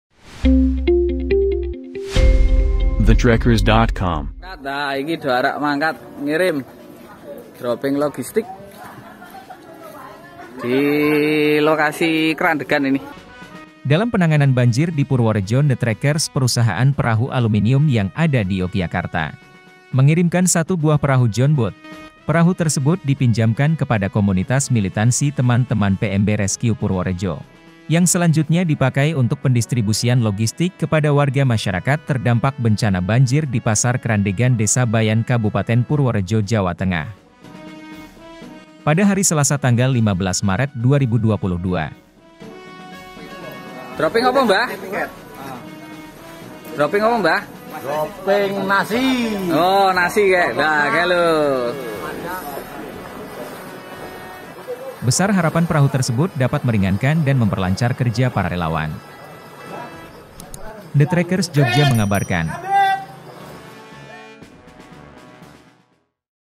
Perahu Aluminium untuk Banjir Purworejo
Pada hari Selasa (15/03/2020) Thetrekkers  Mengirimkan 1 buah perahu jonboat, perahu tersebut dipinjamkan kepada komunitas militansi teman teman pmb rescue Purworejo yang selanjutnya dipakai untuk pendistribusian logistik kepada warga masyarakat terdampak bencana banjir dipasar krandegan desa bayan kab purworejo jawa tengah. Besar harapan perahu tersebut dapat meringankan dan memperlancar kerja para relawan